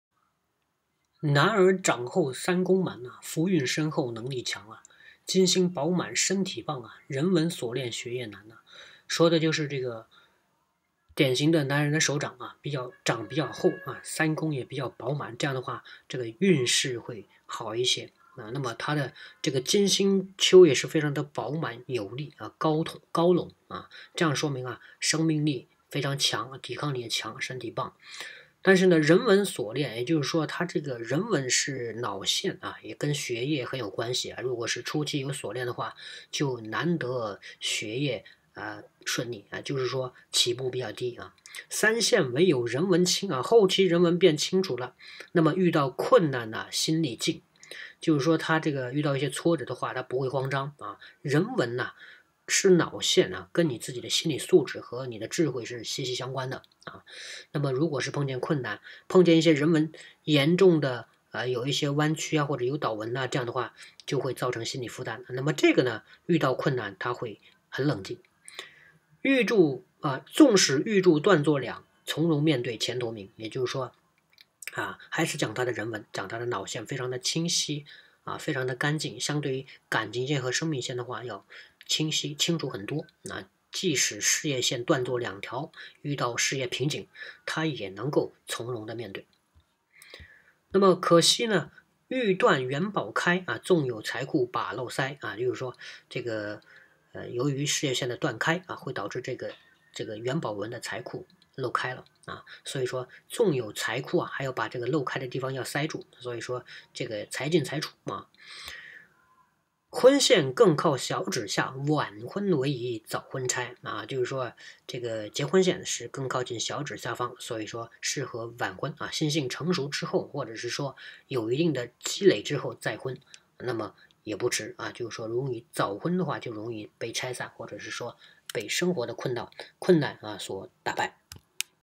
适合晚婚的手相，这样的男人前途无量！有好姻缘！
师傅微❤️：tianyangxuanji
男儿掌厚三宫满，
福运深厚能力强。
金星饱满身体棒，
人纹锁链学业难。（绿色圈）

三线唯有人纹清，
遇到困难心里静。
纵使玉柱断作两，（蓝色）
从容面对前途明。

可惜玉断元宝开，
纵有财库把漏塞。
婚线更靠小指下，（黄色）
晚婚为宜早婚拆。